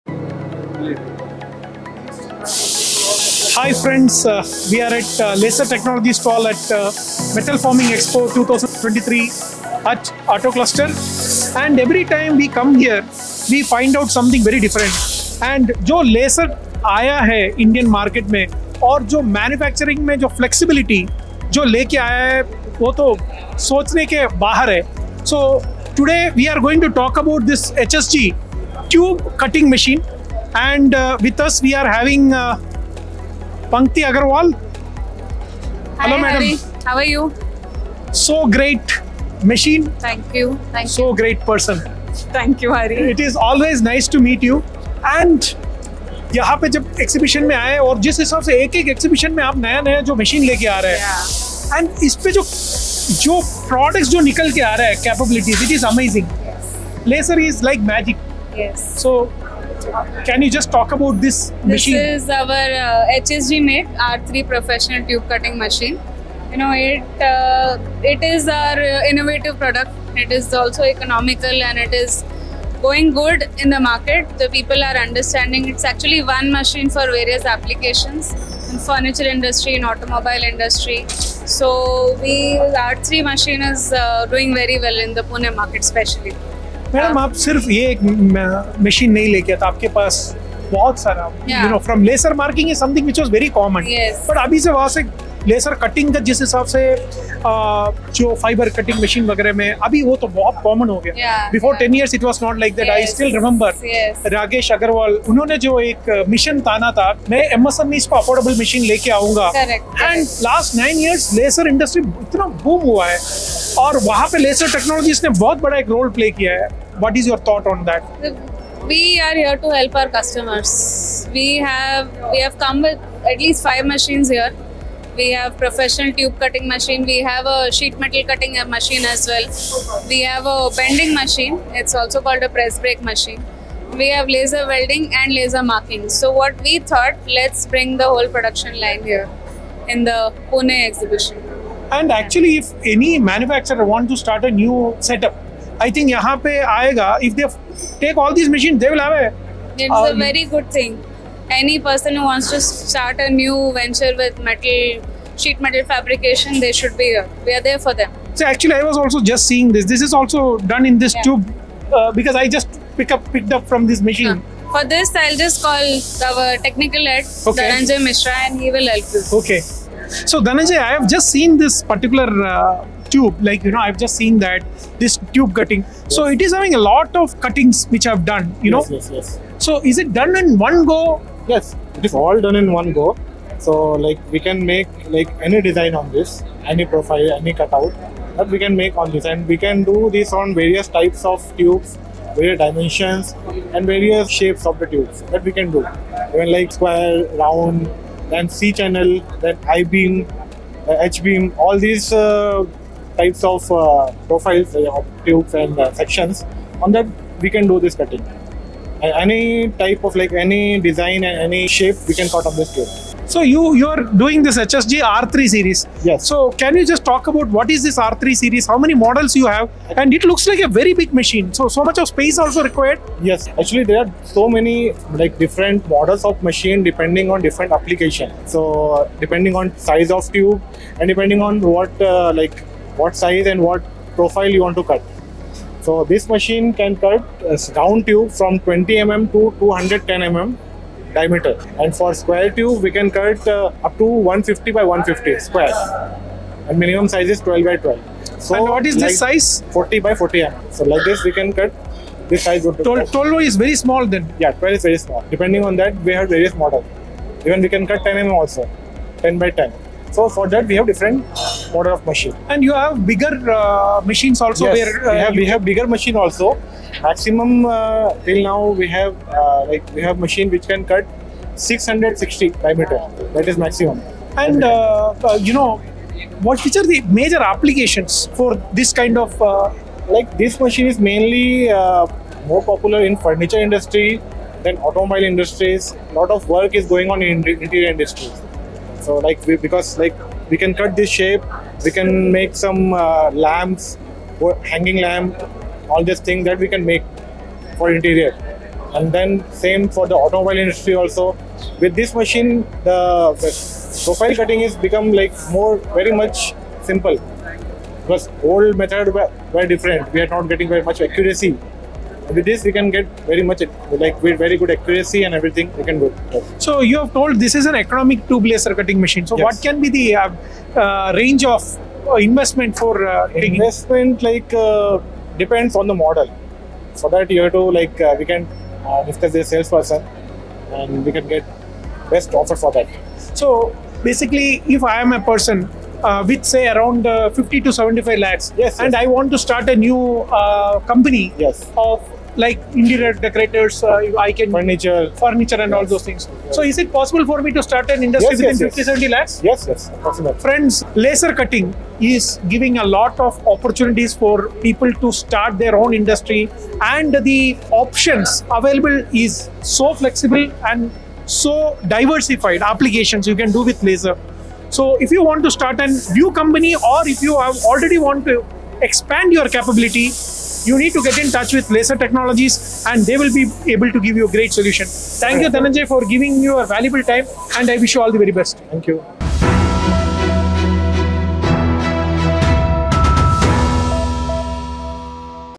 Affordable Laser Technologies for Indian Manufacturing | Metal Forming Expo 2023 | Machine Maker
Laser Technologies, the torchbearer for revolutionizing the Indian manufacturing scene, especially for our diligent MSME sector, once again made waves at the Metal Forming Expo 2023! The company showcased its innovative R3 / R3 PLUS SERIEs economical Tube Laser Cutting Machine (1500W - 4000W) – a creation aligned perfectly with their vision of affordability and advanced technology.

📢 Hari Shanker, the keen-eyed Managing Editor of Machine Maker, dives deep into a conversation with Pankti Agarwal, the dynamic Director of Laser Technologies. They unravel the journey of a decade, under the leadership of Rakesh Agarwal, and how they're scripting the future with machines powered by HSG.

Dhananjay Mishra throws light on the cutting-edge specs of the R3 / R3 PLUS SERIEs machine, from its unique Flying Cutting and Space Leapfrog features to its convenient shipping in a 40HQ Standard Container. And for those eyeing to elevate their production game? The machine offers Optional Automatic Loading and Unloading Device and even Laser-proof Glass!

Don't miss out! Dive into the full interview to discover how Laser Technologies is reshaping the contours of Indian manufacturing. 🛠✨